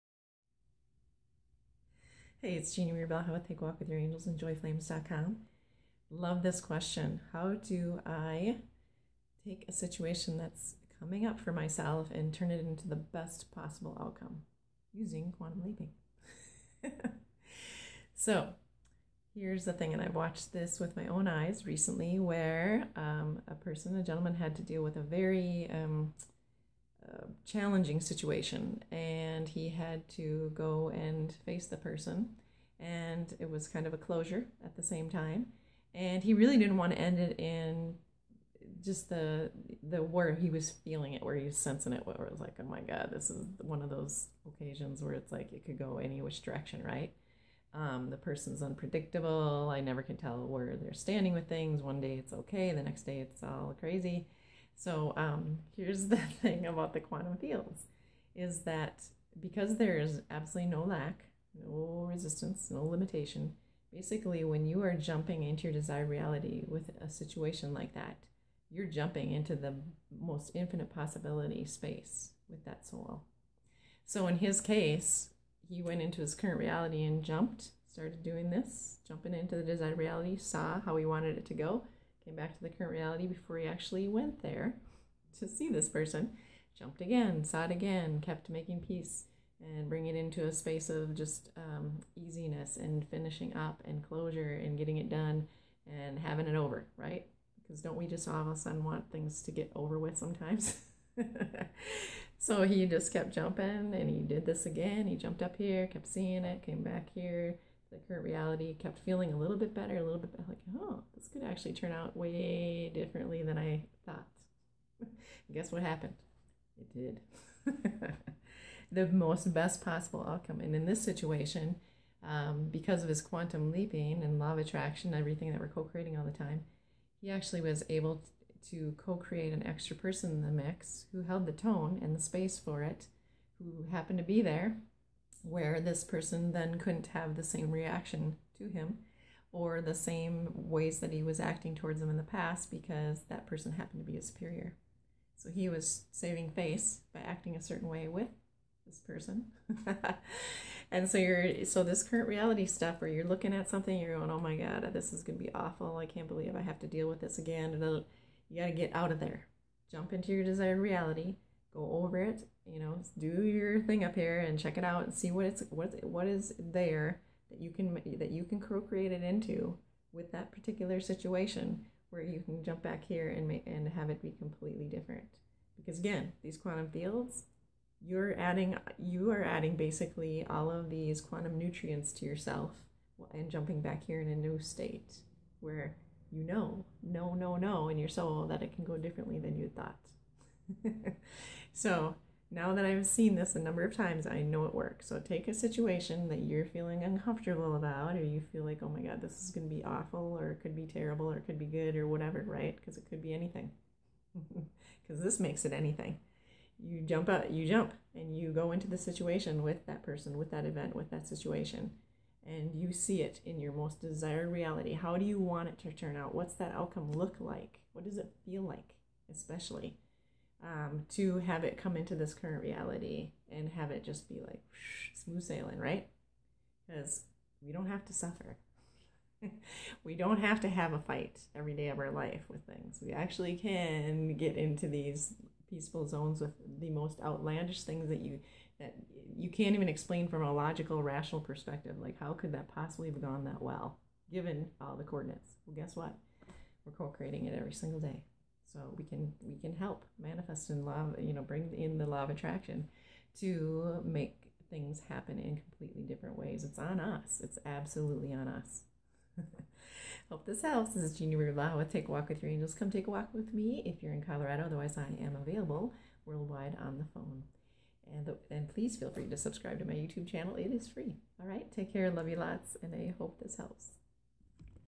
"I've A Challenging Situation Coming Up; How Can I Quantum Leap It To The Best Possible Outcome?"
Jeannie shares how you can take any situation or event & quantum leap with it to a best possible outcome for all involved. You may be amazed by how much say you really do have as a co-creator of your whole life!